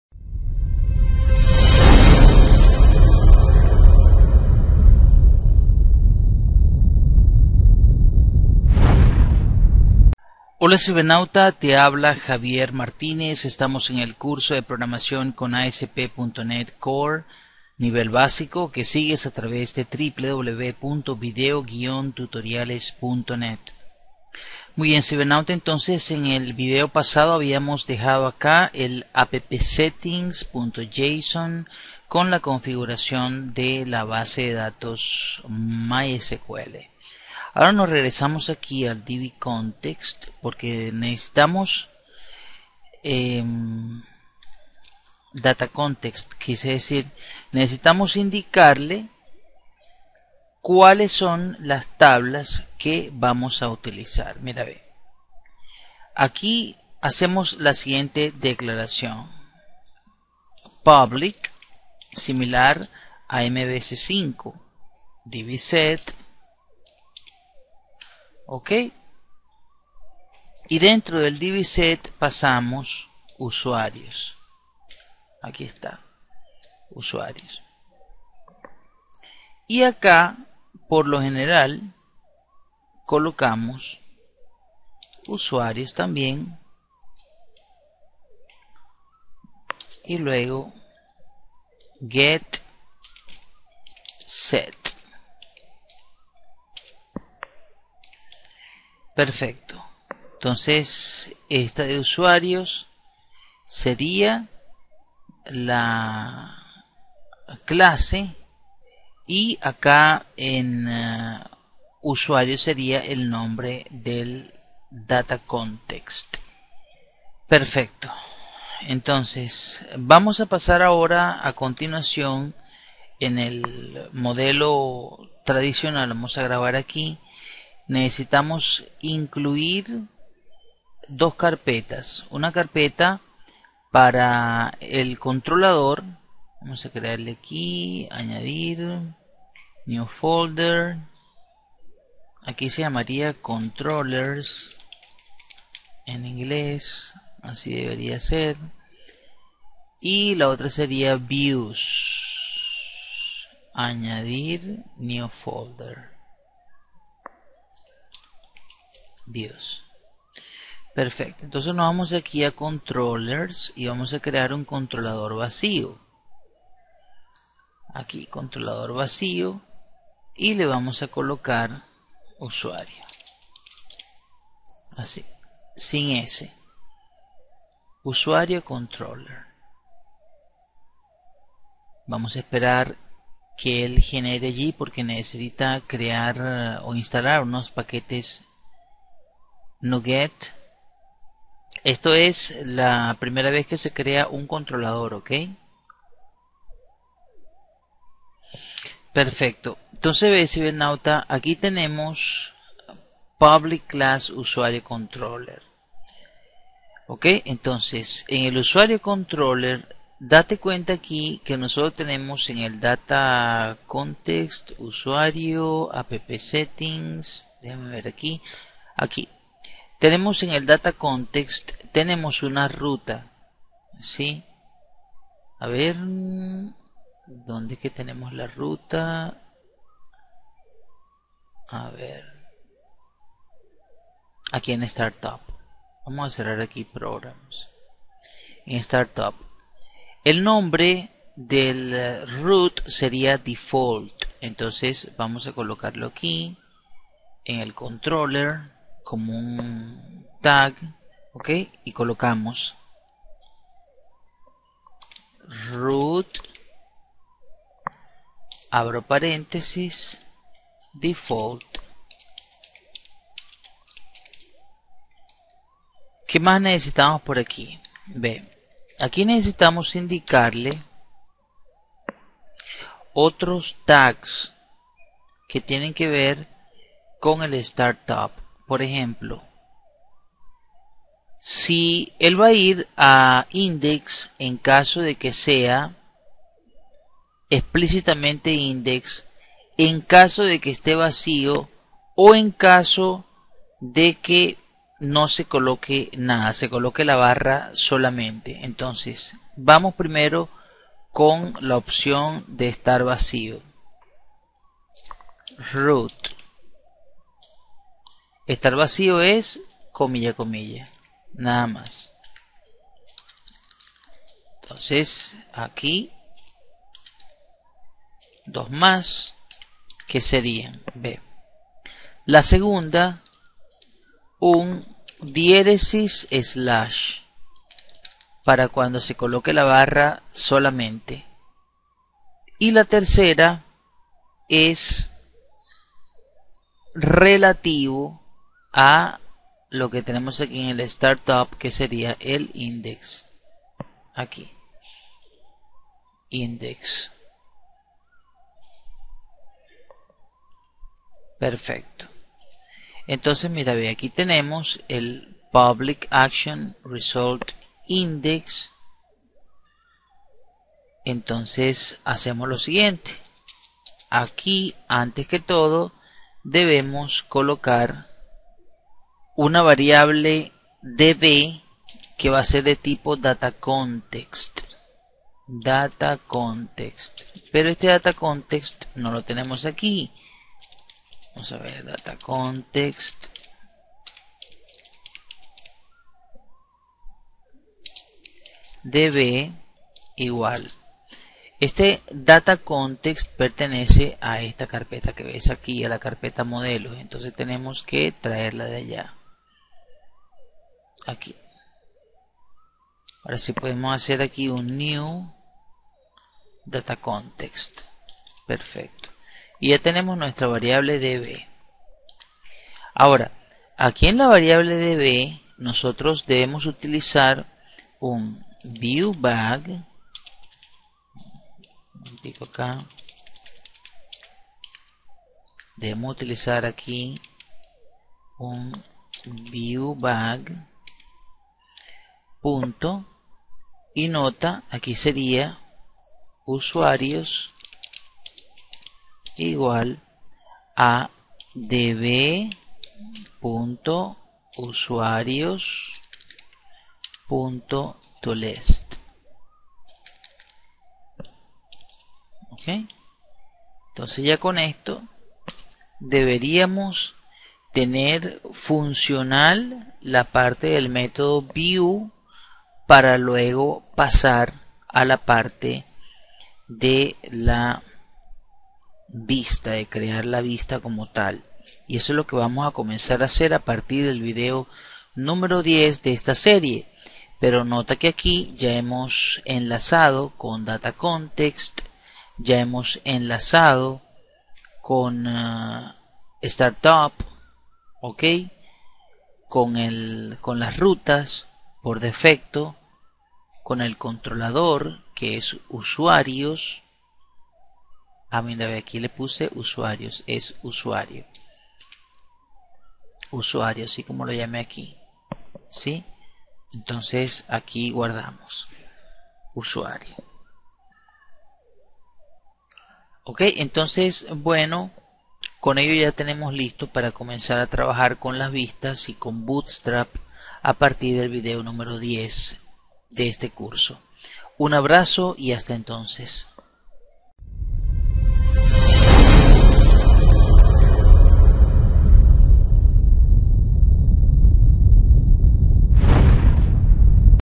ASP NET CORE CON MYSQL - CREANDO EL CONTROLADOR PARA NUESTRA TABLA USUARIOS -VIDEO 09
Ahora pasamos a crear la carpeta Controllers de nuestro proyecto y el Controlador para la tabla Usuarios. Usando los DataAnnotations modificamos nuestra acción principal Index para controlar las rutas respectivas.